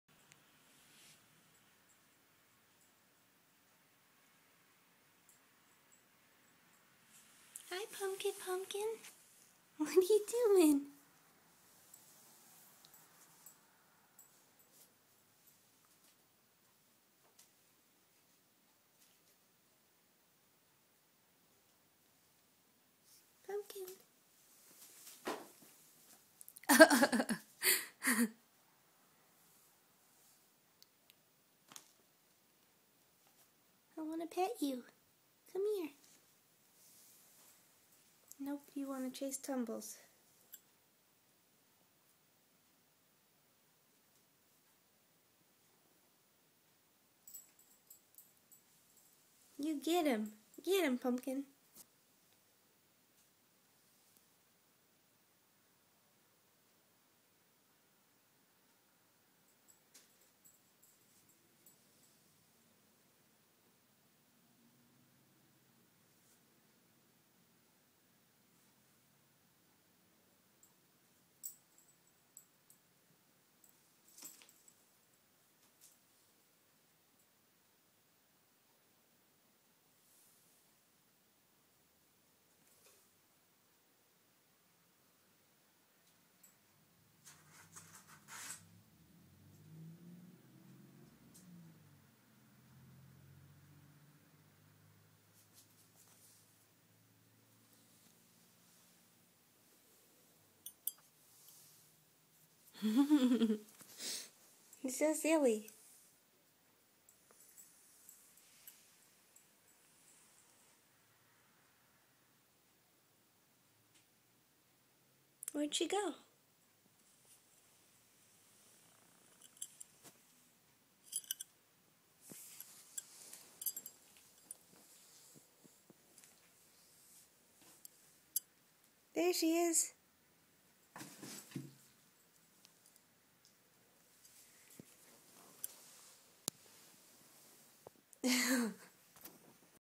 Pumpkin & Tumbles
Already becoming best pals, though they just met. Not sure if Pumpkin does a couple binkys in this video? I'm still so new to bunnies!